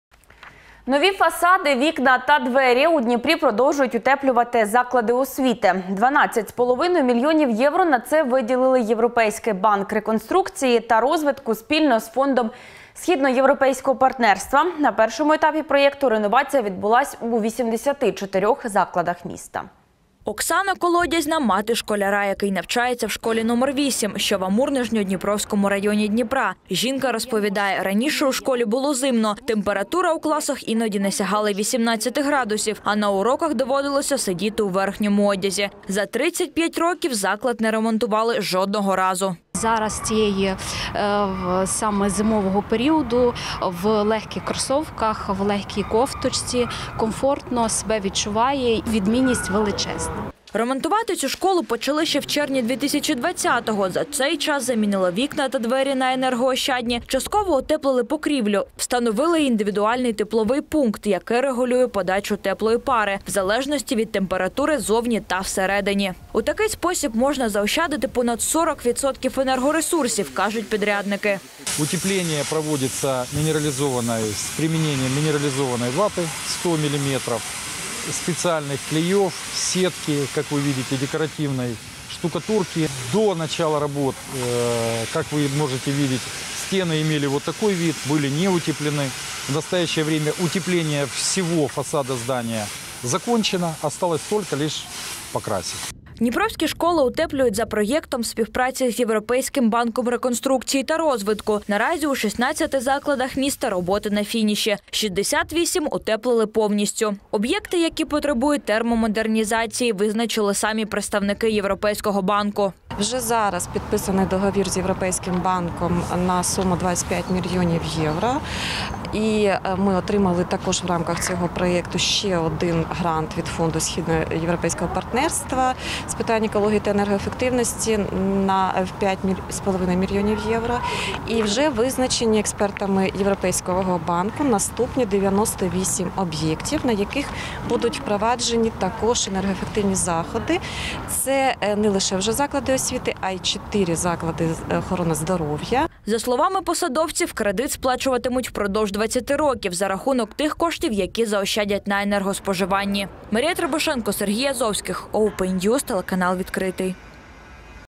Школи Дніпра отримали 12,5 мільйонів євро на утеплення
У Дніпрі продовжують утеплювати заклади освіти. 12,5 мільйонів євро на це виділили Європейський банк реконструкції та розвитку спільно з Фондом Східноєвропейського партнерства.